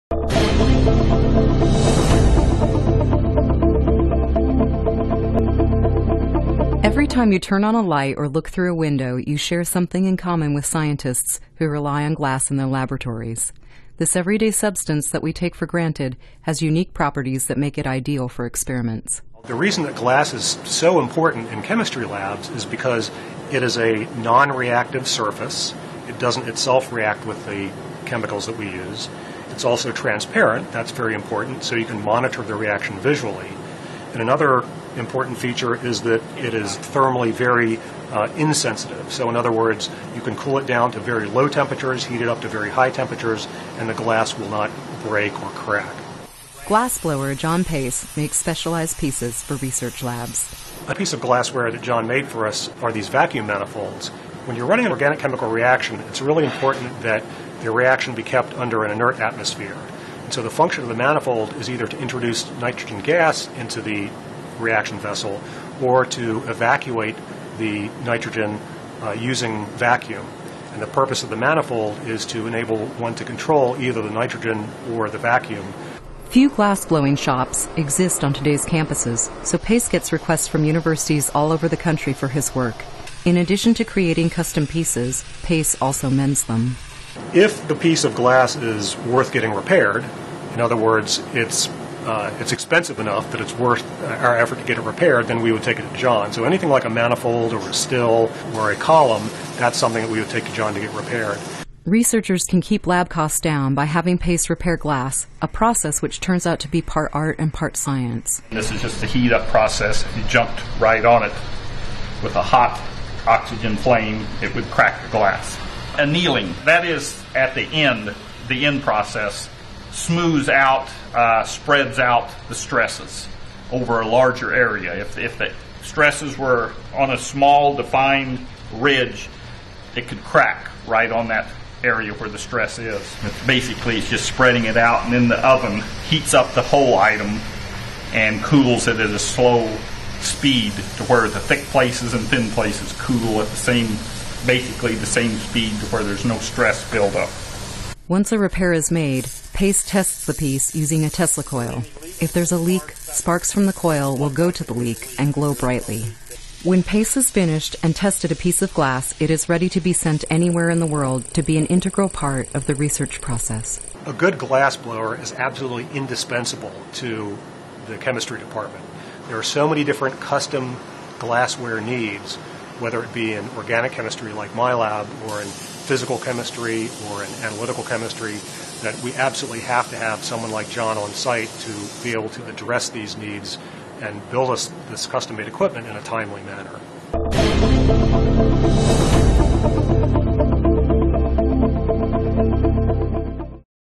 A Glass Act
Master scientific glassblower John Pace works in Research Support and Sponsored programs and helps keep scientific laboratories running by designing custom-made pieces and repairing costly glassware when it breaks.

Provided by the University of Arkansas